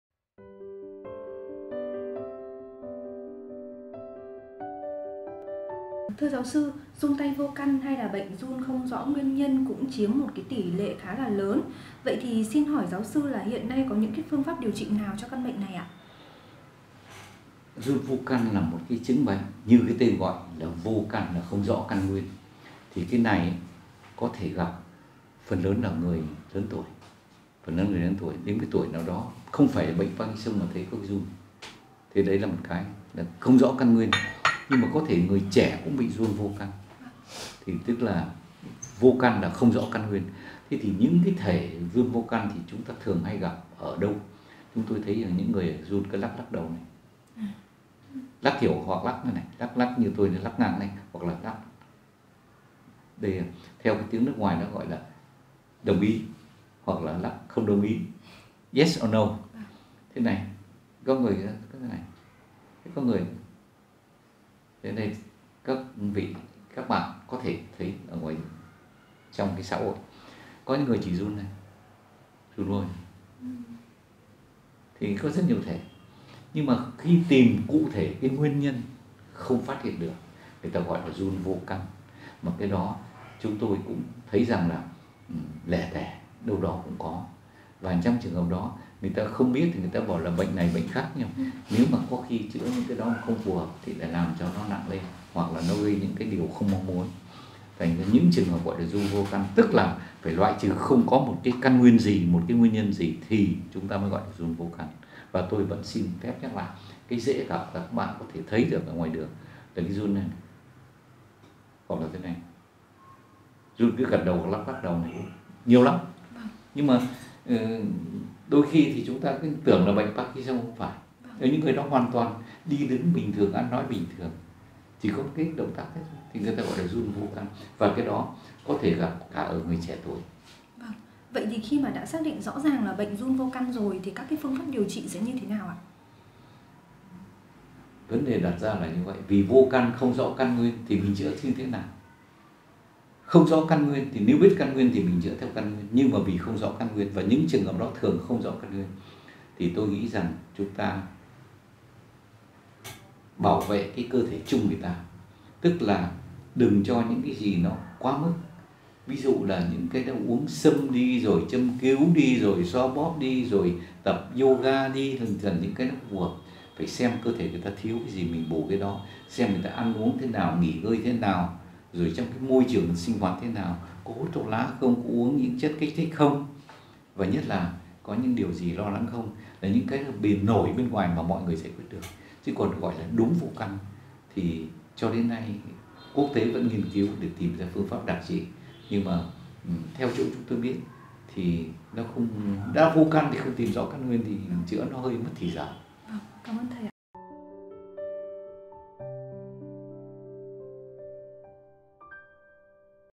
Run vô căn là bệnh gì, chữa trị như thế nào
Run vô căn là một chứng bệnh như tên gọi của nó, vô căn là không rõ căn nguyên. Vì không rõ nguyên nhân nên việc điều trị căn bệnh này cũng gặp nhiều khó khăn.

Do không có loại thuốc nào đặc trị bệnh run vô căn mà tùy thuộc vào thấy thuốc khi thăm khám, nếu phát hiện cơ thể người bệnh thiếu chất gì, sẽ cho bổ sung các chất đó. Thế nhưng theo Gs.Ts Lê Đức Hinh điều quan trọng là người bệnh cần có chế độ ăn uống hợp lý, nghỉ ngơi đầy đủ và tránh những muộn phiền lo lắng để giúp làm giảm run.